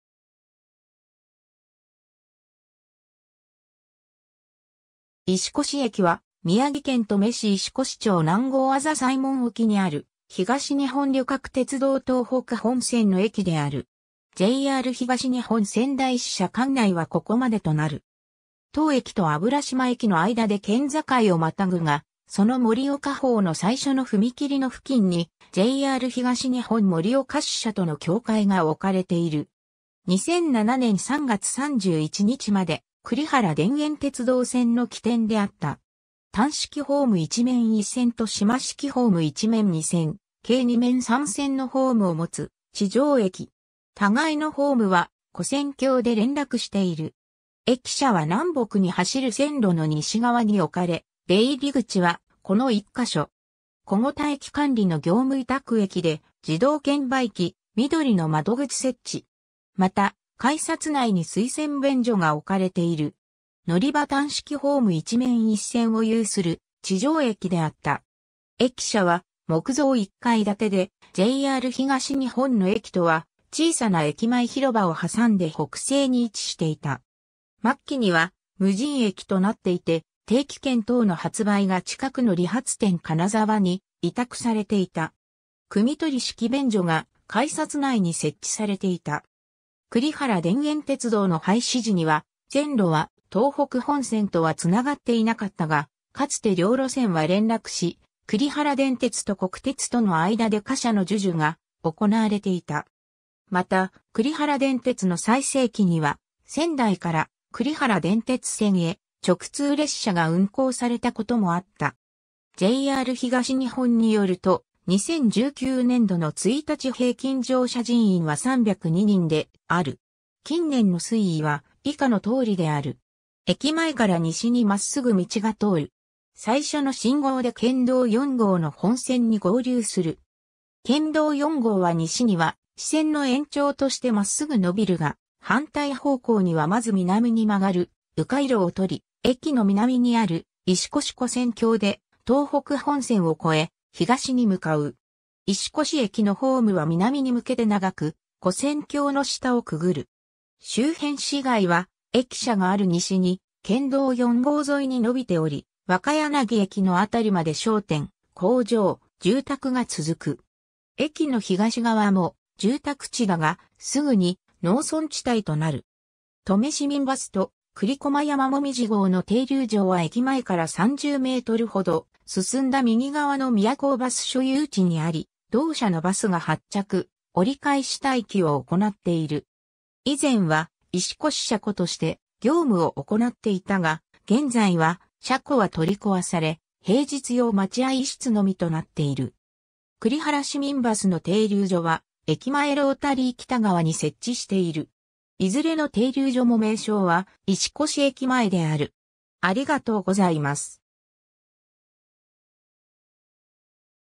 石越駅
石越駅（いしこしえき）は、宮城県登米市石越町南郷字西門沖（みなみごうあざにしもんおき）にある、東日本旅客鉄道（JR東日本）東北本線の駅である。
 JR東日本仙台支社管内はここまでとなる。
当駅と油島駅の間で県境を跨ぐが、その盛岡方の最初の踏切の付近（下原田踏切、東京起点425km地点）にJR東日本盛岡支社との境界が置かれている。
2007年（平成19年）3月31日までくりはら田園鉄道線の起点であった。
 単式ホーム1面1線と島式ホーム1面2線、計2面3線のホームを持つ地上駅。
 駅舎は南北に走る線路の西側におかれ、出入口はこの1箇所。
小牛田駅管理の業務委託駅で、自動券売機・みどりの窓口（朝晩は無人）設置。
また、改札内に水洗便所が置かれている。
 のりば 単式ホーム1面1線を有する地上駅であった。
駅舎は木造一階建てで、JR東日本の駅とは小さな駅前広場をはさんで北西に位置していた。
末期には無人駅となっていて、定期券等の発売が近くの理髪店カナザワに委託されていた。
汲み取り式便所が改札内に設置されていた。
 くりはら田園鉄道の廃止時には、線路は東北本線とは繋がっていなかったが、かつて両路線は連絡し、栗原電鉄と国鉄との間で貨車の授受が行われていた。
また、栗原電鉄の最盛期には、仙台から栗原電鉄線へ直通列車が運行されたこともあった。
 JR東日本によると、2019年度（令和元年度）の1日平均乗車人員は302人である。
 駅前から西にまっすぐ道が通る（宮城県道4号中田栗駒線の支線）。
最初の信号で県道4号の本線に合流する。
県道4号は西には支線の延長としてまっすぐ伸びるが、反対方向にはまず南に曲がる迂回路をとり、駅の南にある石越跨線橋で東北本線を越え、東に向かう。
石越駅のホームは南に向けて長く、跨線橋の下をくぐる。
 周辺市街は駅舎がある西に、県道4号沿いに伸びており、若柳駅のあたりまで商店、工場、住宅が続く。
駅の東側も住宅地だが、すぐに農村地帯となる。
 登米市民バスと栗駒山紅葉号の停留所は駅前から30mほど進んだ右側のミヤコーバス所有地にあり、同社のバスが発着・折返し待機を行っている。
以前は石越車庫として業務を行っていたが、現在は車庫は取り壊され、平日用待合室のみとなっている。
栗原市民バスの停留所は駅前ロータリー北側に設置している。
いずれの停留所も名称は「石越駅前」である。